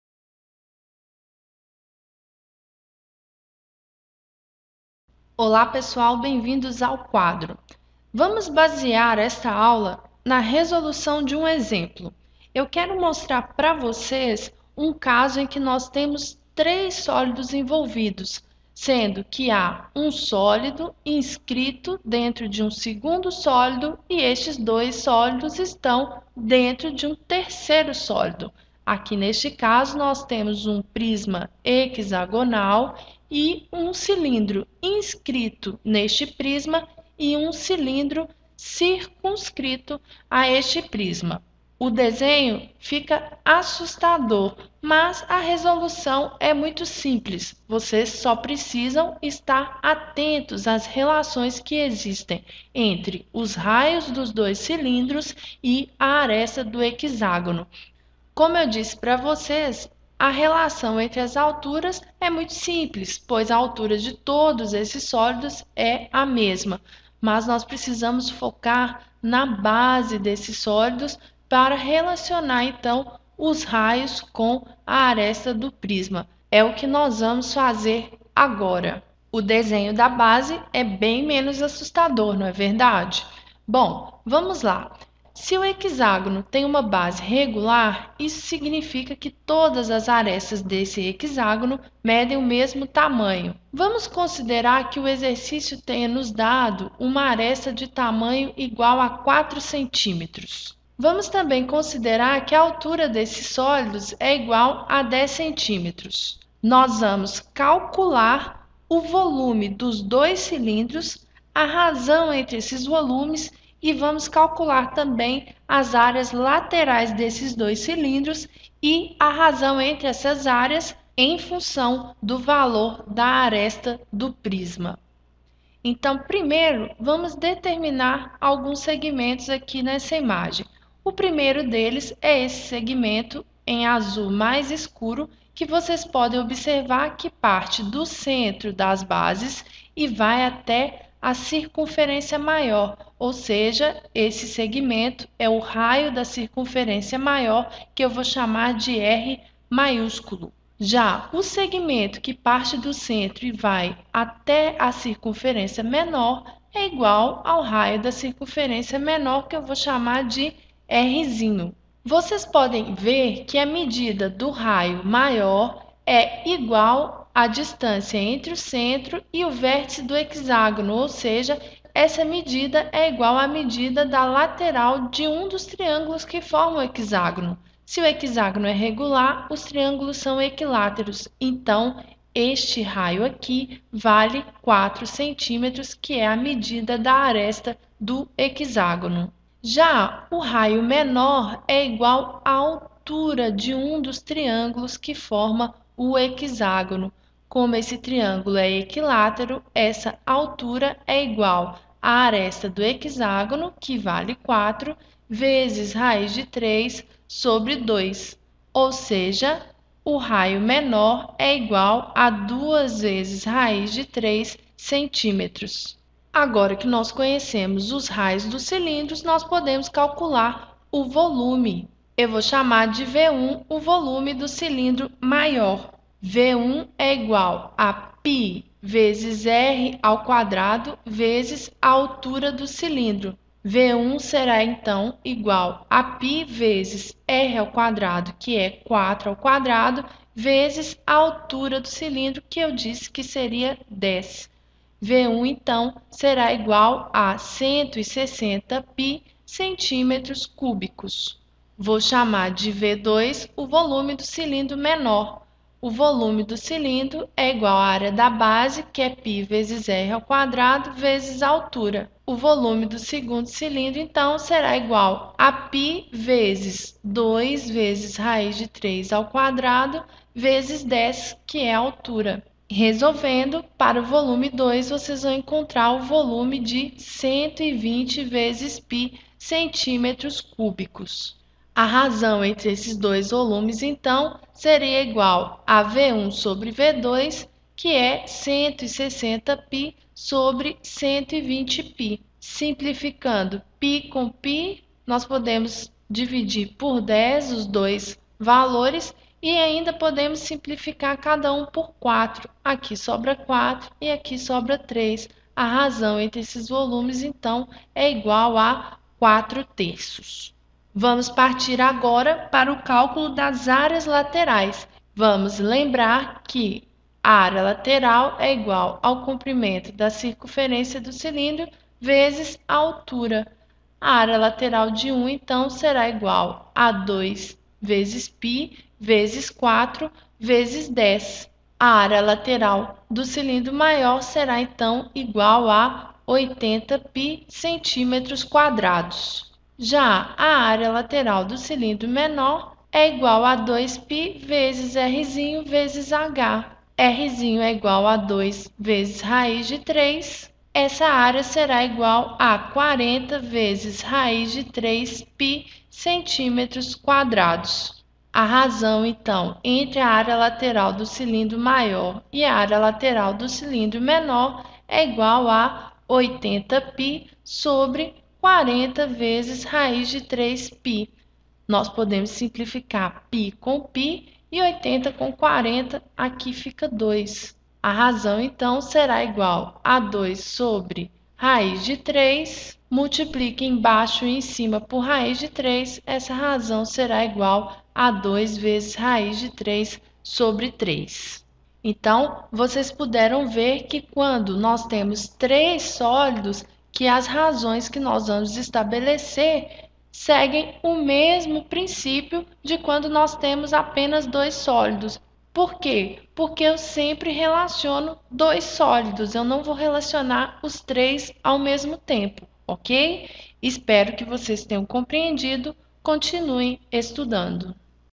Dois cilindros e um prisma
Veja como relacionar raios e arestas quando um prisma circunscrito a um cilindro está inscrito em um segundo cilindro.